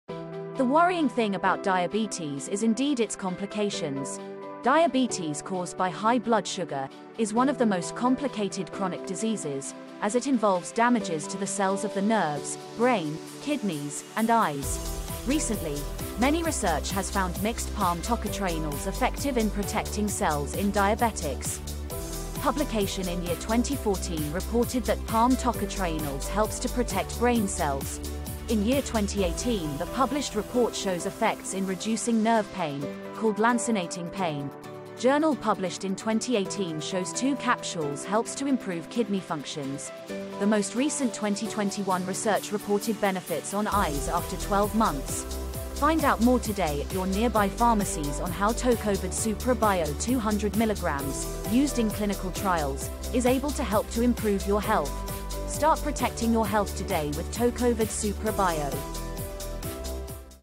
Tocovid Suprabio 200mg for diabetes complication. Helps protect cells
📢 Research scientists found that Tocovid Suprabio™ 200mg with a unique 7 Actives Mixed from red palm oil extract, plus the patented Suprabio™ absorption system, shows proven benefits in protecting cells of the
✔ Brain,
✔Kidney,
✔Nerves
✔Liver,
and helps people with underlying diseases like DIABETES and HYPERTENSION

You can purchase from Hovid Nutriworld at Lazada at SPECIAL DEAL:

Ask at your nearest pharmacy today for Tocovid Suprabio 200mg, the 100% Original Palm Tocotrienols used in clinical trials by HOVID